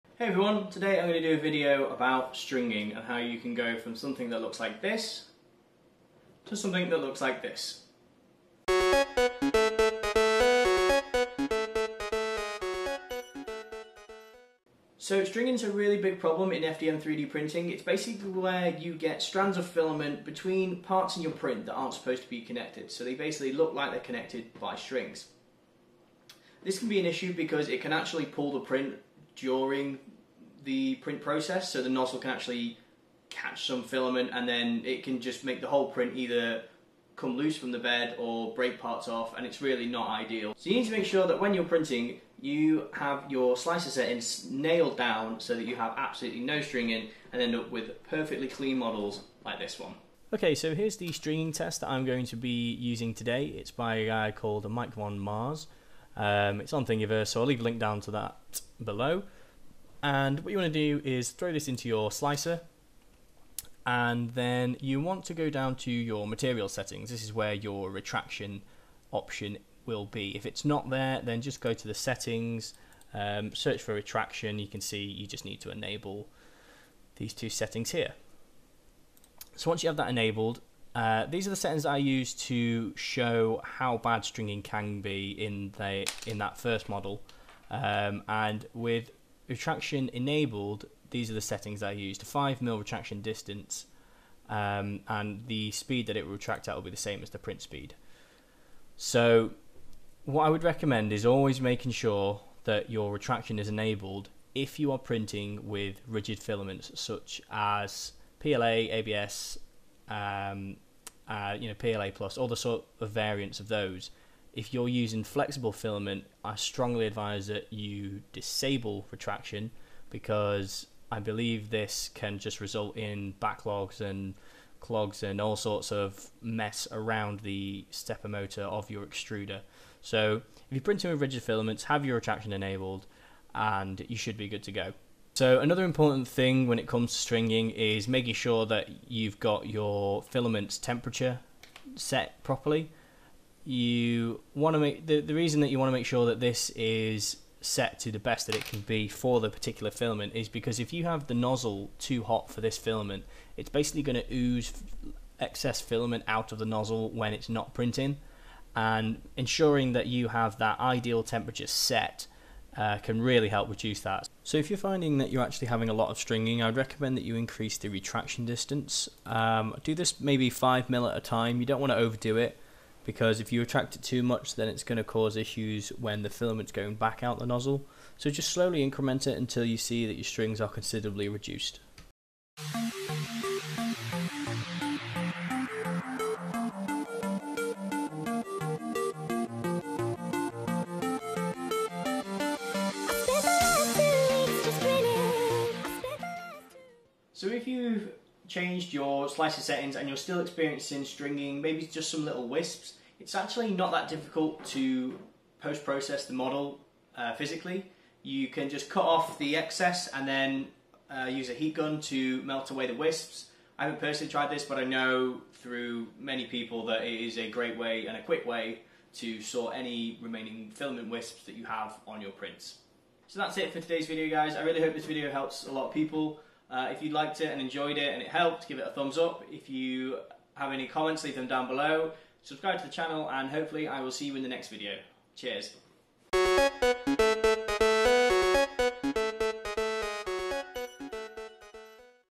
A Beginners Guide To 3D Printing - How To Combat Stringing!
In this beginners guide video we will talk about how to combat stringy prints!

***LINKS***